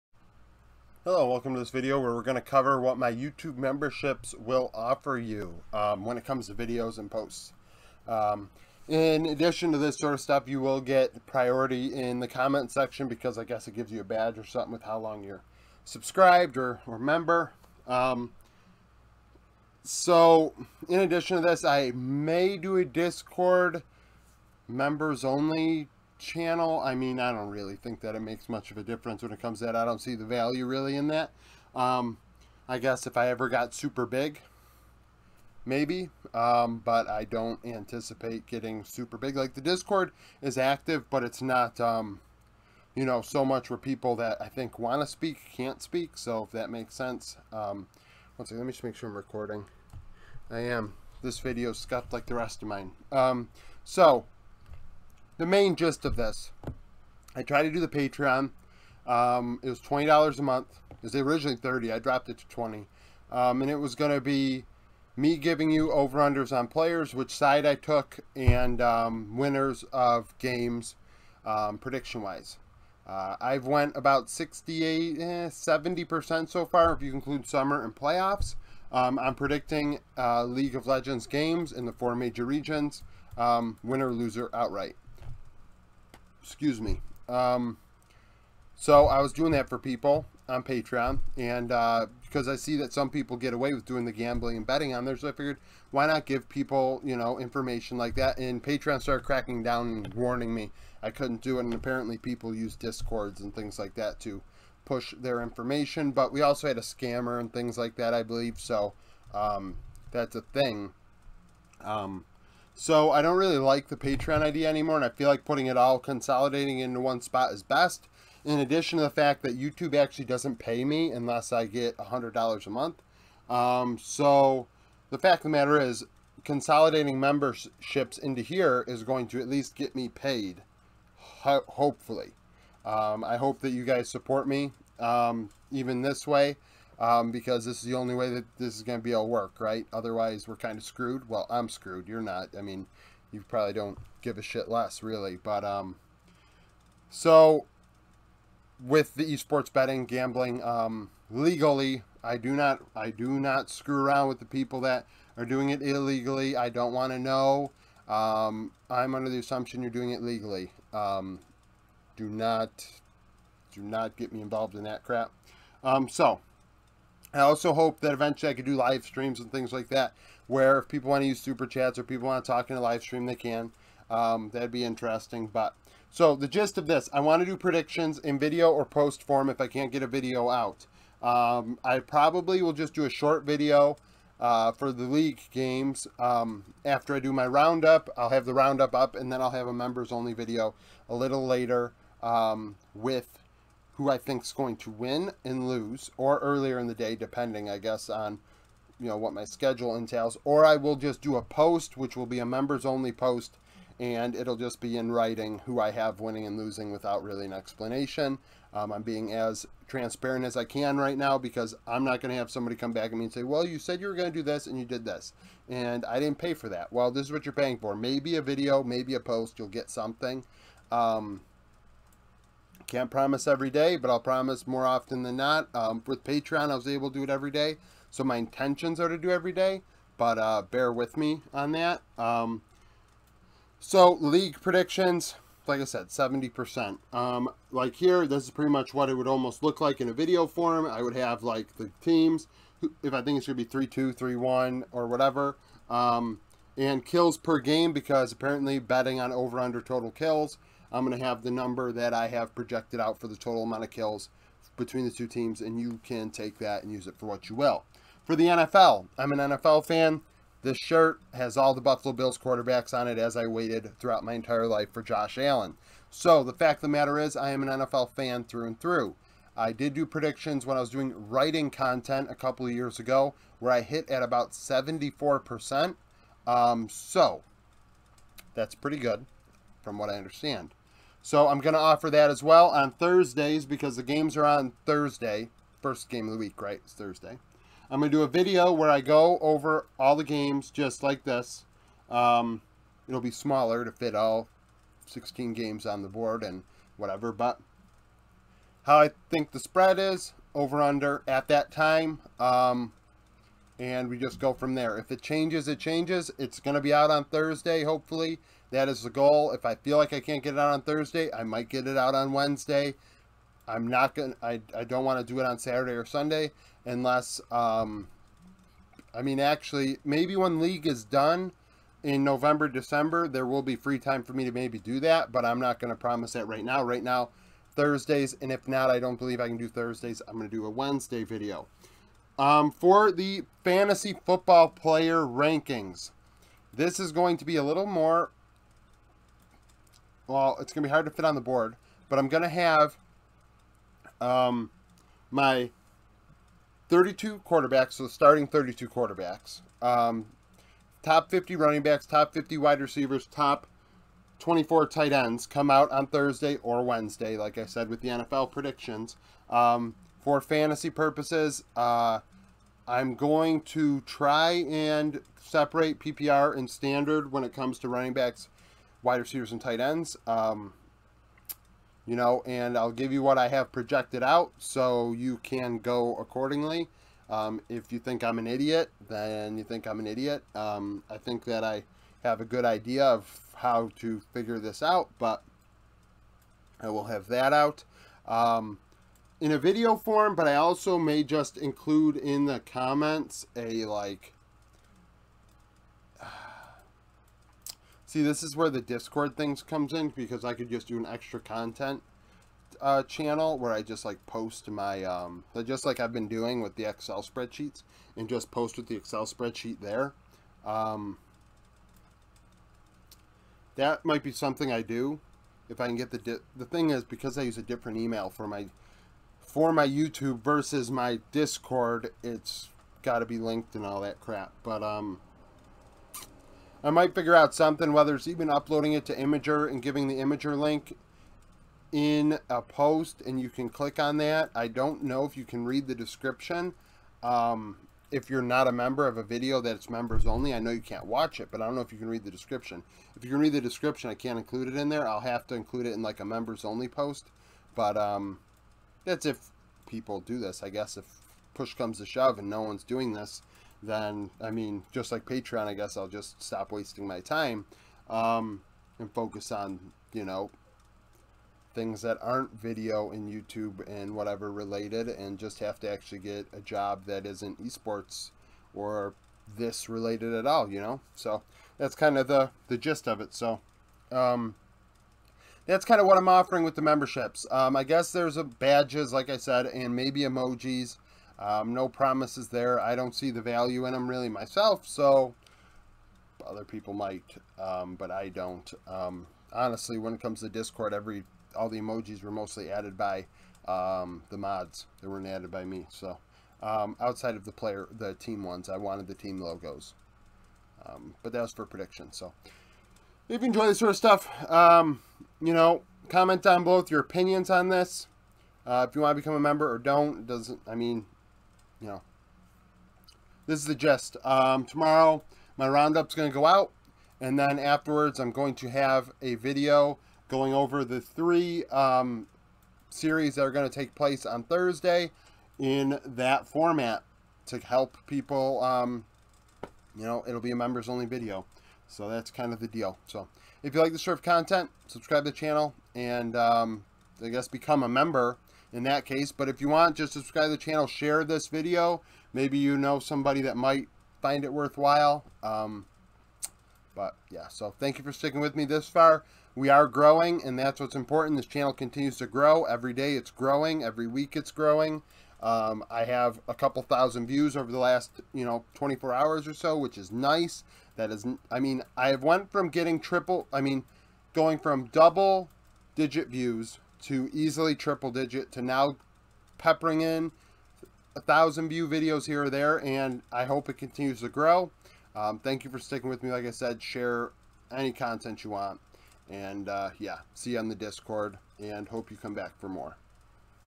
Additional Content and YouTube Memberships
In this video I give you an overview of some additional, members-only content I intend on putting out.

I will be offering badges and etc. as part of the membership and reply to your comments in the comment section of the videos.

If you partake in legal esports, and/or NFL betting as well as fantasy football (American football) this may be for you.